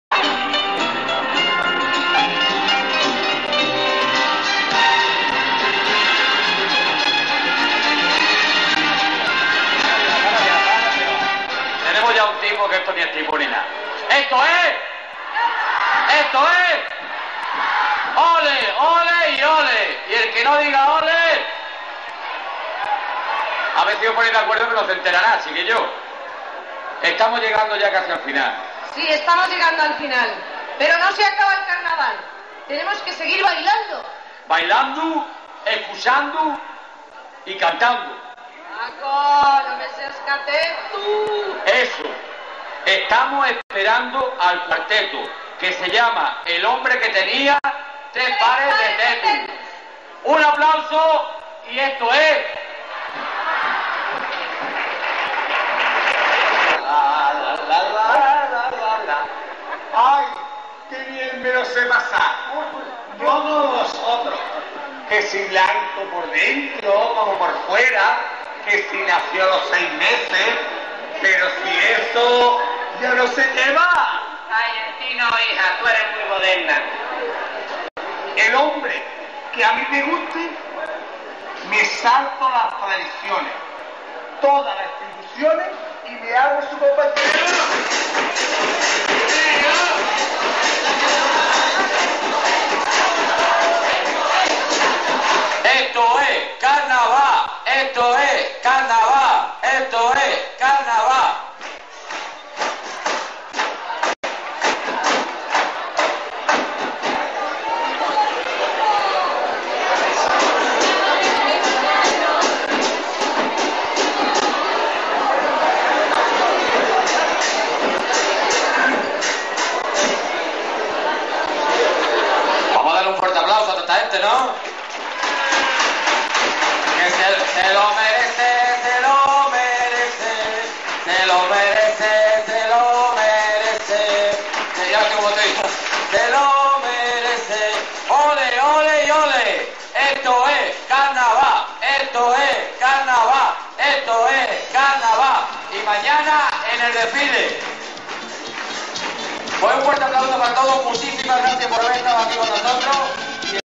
NERJA - CARNAVAL 1990 - ENTREGA DE PREMIOS
Actuación del cuartetus el Hombre que tenia tres pares de tetus
Entregas de premios
Presentado por Carmrn La Nenona y Paco Haro